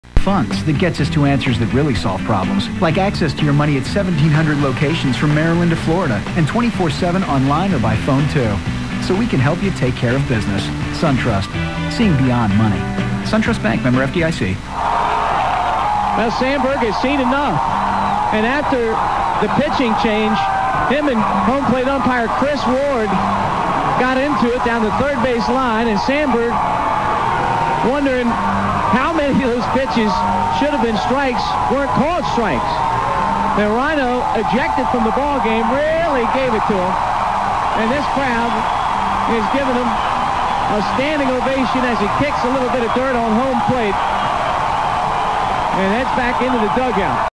Sandberg Ejected
Watch Ryno give the Umps a piece of his mind after some "questionable calles" in the 9th inning on July 3, 2009 as the Smokies take on the Huntsville Stars at Smokies Park...